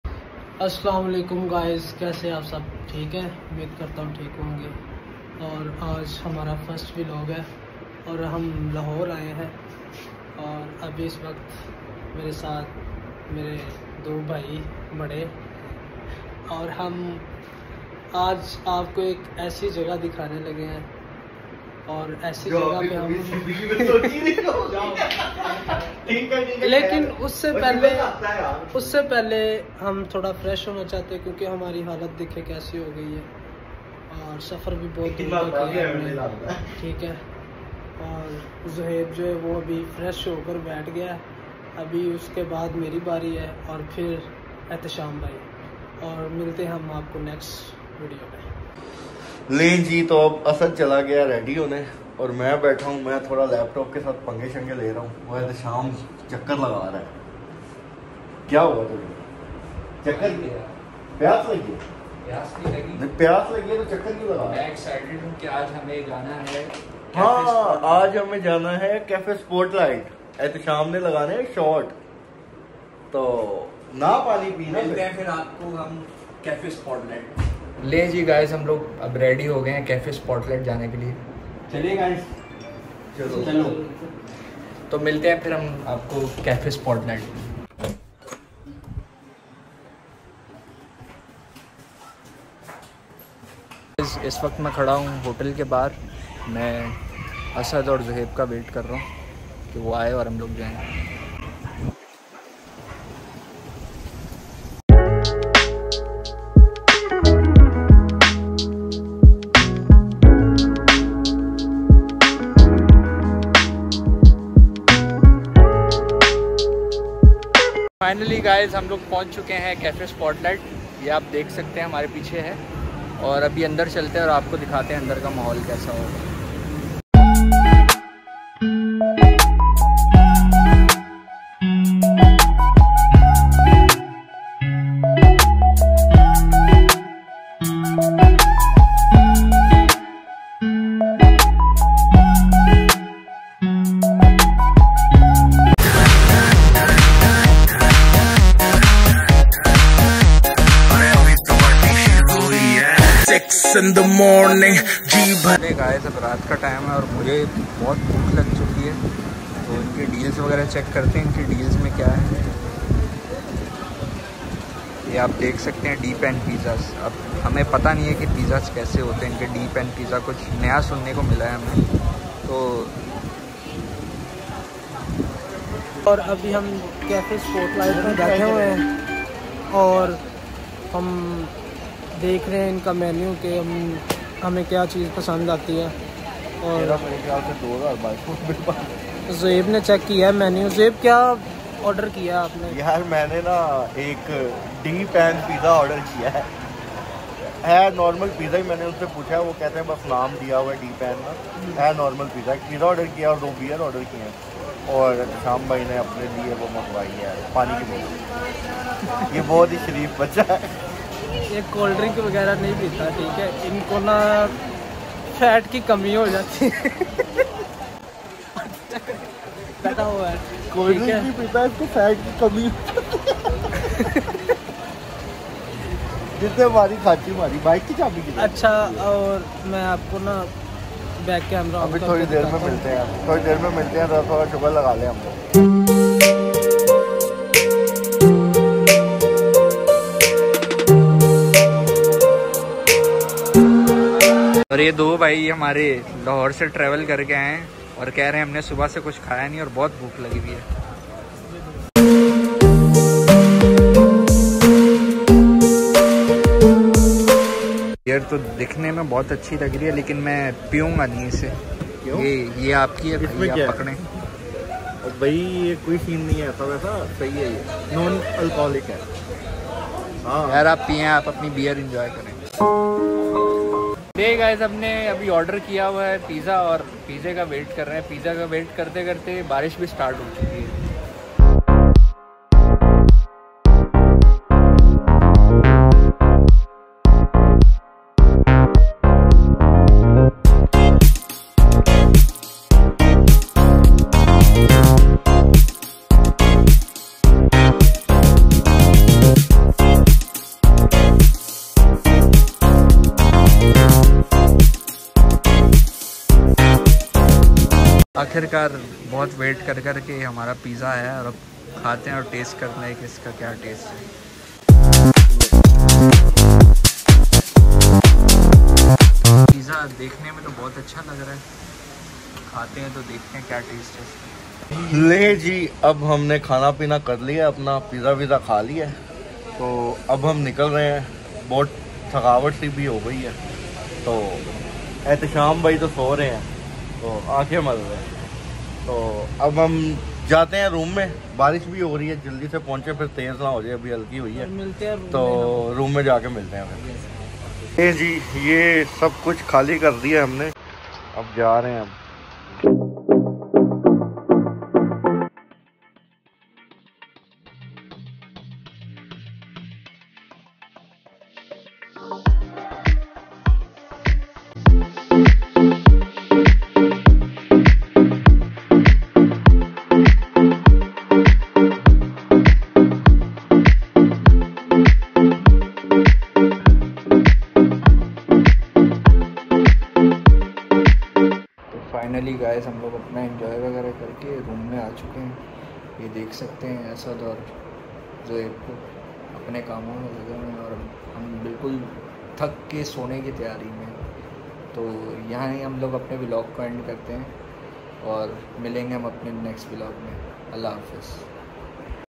Vlog 1 - cafe spotlight - fun with friends - best time - Its Ehtesham
Cafe spotlight is one of the best place for fun and hangout with friends. Services are best taste is best. One of the most attractive thing is the juice bar you can enjoy the fire work as have captured in my video. If you wanna visit that place please find the attached location in my video discrition.

Cafe Spotlight, 43 Service Rd, Airline Society, Lahore, Punjab, Pakistan